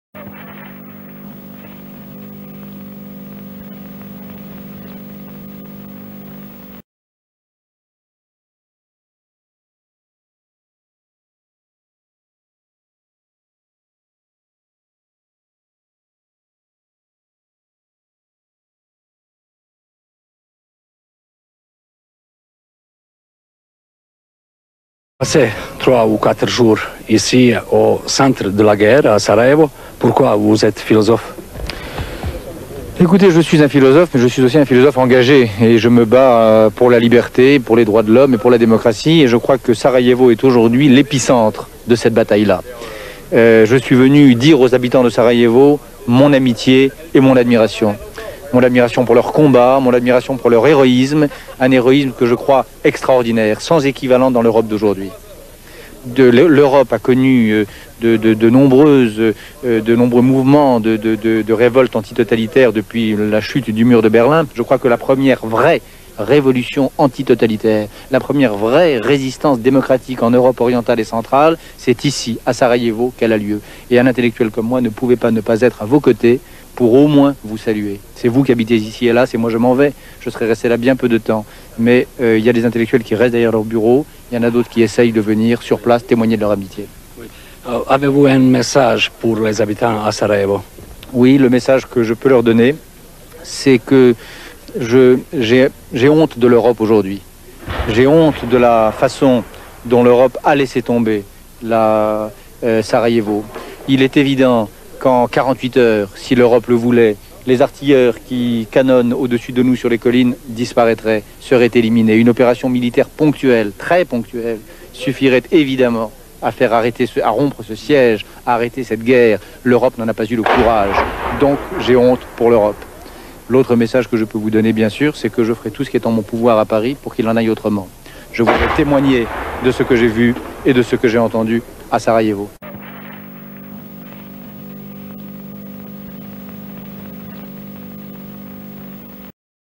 Rječnik opsade: BERNARD HENRY LEVY
“Bosna!” doesn’t pretend to be anything other than what it is, an inflammatory, highly partisan cry for help. Let it not be said, Mr. Levy pleads in the film’s final words, that “Europe died at Sarajevo.”